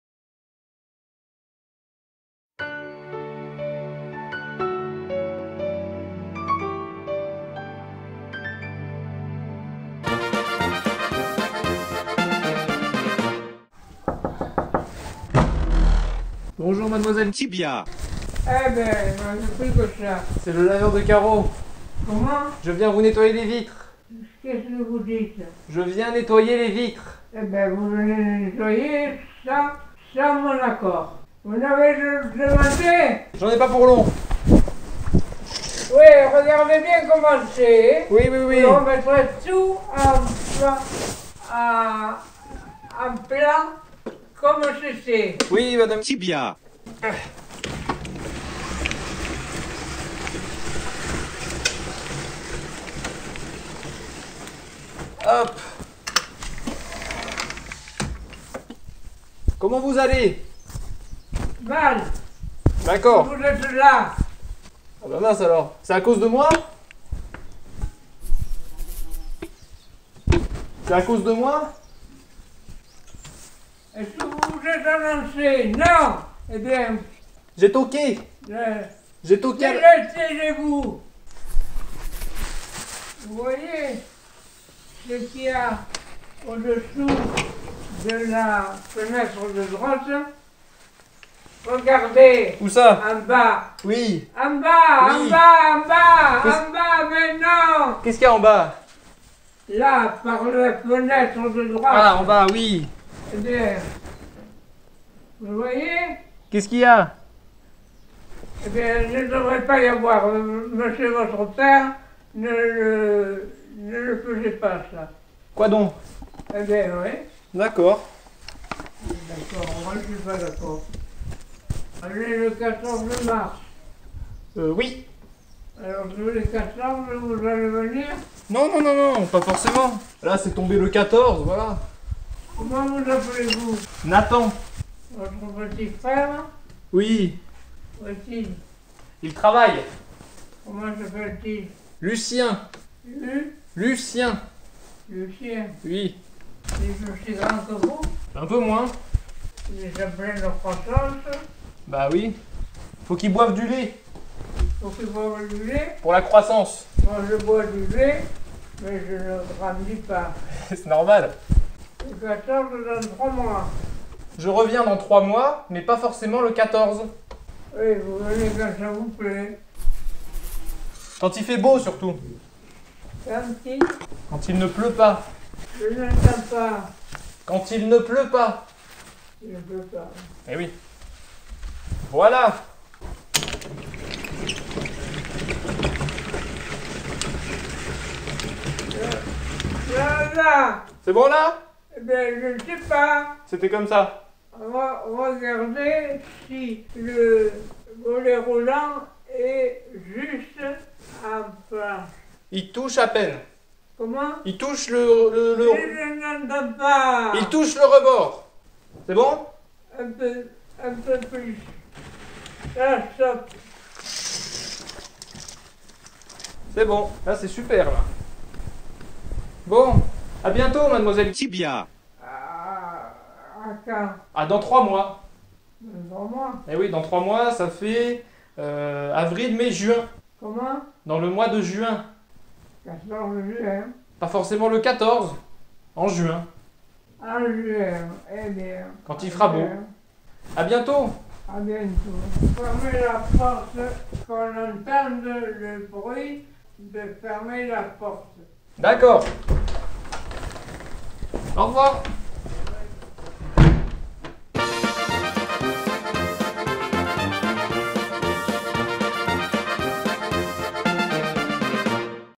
La formidable mademoiselle Tibia - EPISODE 2 [Animation 2D]
L'épisode 1 vous avait plu, voici la suite des aventures de la formidable mademoiselle Tibia !.
Cette courte animation m'a demandé une dizaine d'heure de boulot, alors j'espère vraiment que ça vous a plu, faites le moi savoir par un p'tit pouce bleu, mais surtout par un commentaire ! J'ai vraiment hâte de lire vos réactions !
Et vous pouvez partager ça autour de vous si ça vous a fait rire ! ;)

- Voici en bonus une vieille vidéo Tuto d'animation 2D, qui montre comment j'ai procédé pour réaliser cette animation, même si depuis ma technique a un peu évolué